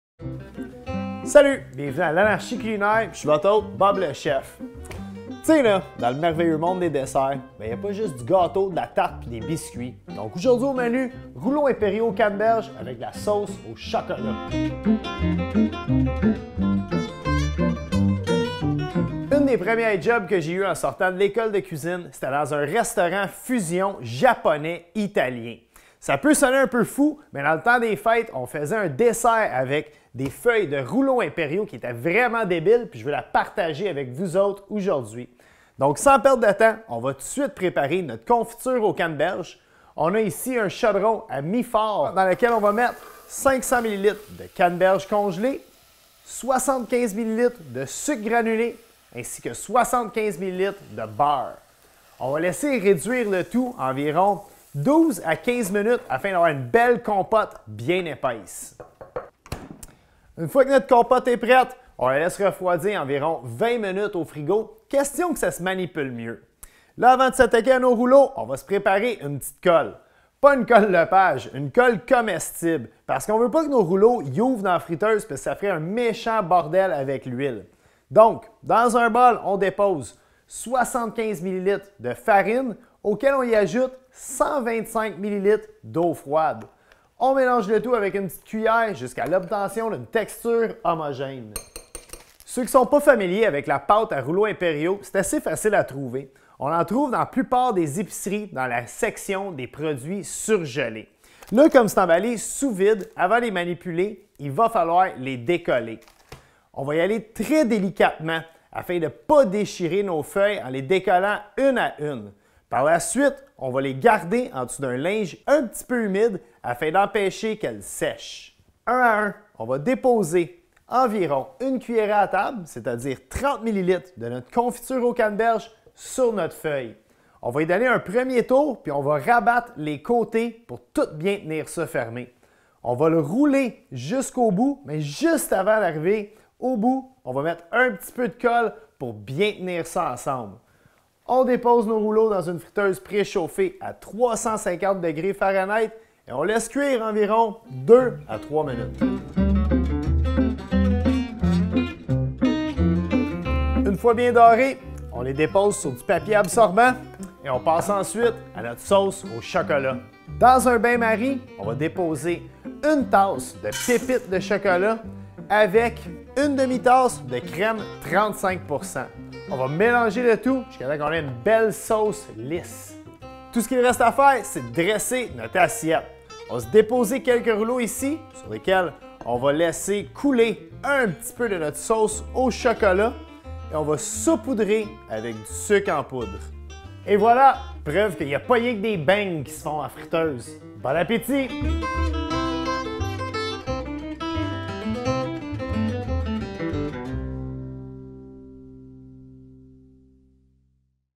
Rouleaux impériaux de canneberges et sauce au chocolat selon Bob le Chef
Parce que dans le merveilleux monde des desserts, il n’y a pas que les tartes, les gâteaux et les biscuits, au menu : rouleaux impériaux aux canneberges et sauce au chocolat.